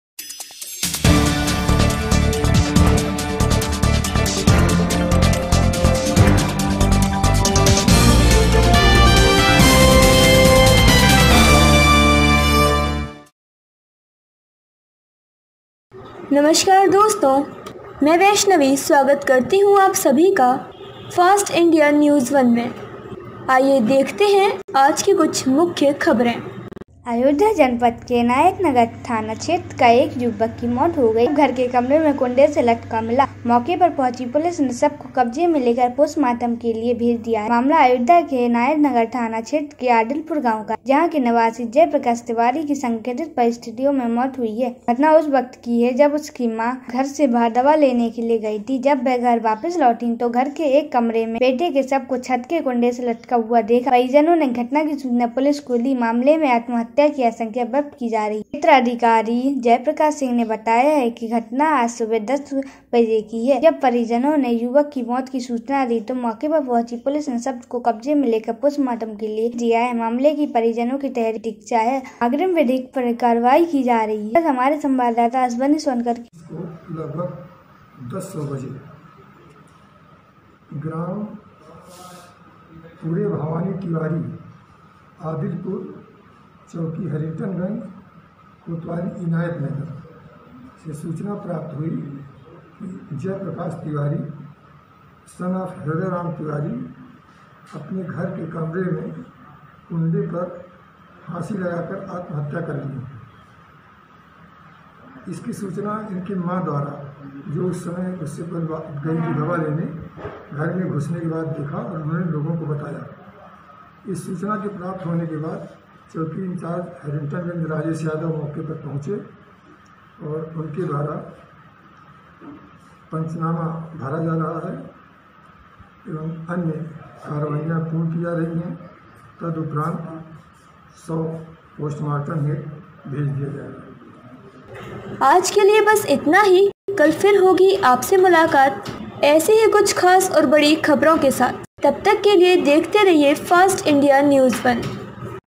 अयोध्या17-05-2020
अयोध्या जनपद के इनायतनगर थाना क्षेत्र में एक युवक की मौत हो गई है. शव घर के कमरे में कुंढे से लटका मिला है।मौके पर पहुंची पुलिस ने शव को कब्जे में लेकर पोस्टमार्टम के लिए मोर्चरी भेज दिया है।मामला अयोध्या के इनायतनगर थाना क्षेत्र के आदिलपुर गांव का है।जहां के निवासी जय प्रकाश तिवारी की संदिग्ध परिस्थितियों में मौत हुई है. घटना उस वक्त हुई जब युवक की मां घर से बाहर दवा लेने गई थी. जब वह घर वापस लौटी तो घर के एक कमरे में बेटे के शव को छत के कुंढे से लटका हुआ देखा. परिजनों ने घटना की सूचना पुलिस को दी. मामले में आत्महत्या की आशंका व्यक्त की जा रही है।क्षेत्राधिकारी मिल्कीपुर जयप्रकाश सिंह ने बताया कि घटना आज सुबह करीब 10:00 बजे की है. जब परिजनों ने युवक की मौत की सूचना दी थी. मौके पर पहुंची पुलिस ने शव को कब्जे में लेकर पोस्टमार्टम के लिए मोर्चरी भेज दिया है. मामले में परिजनों के तहरीर की प्रतीक्षा है. अग्रिम विधिक कार्रवाई की जा रही है.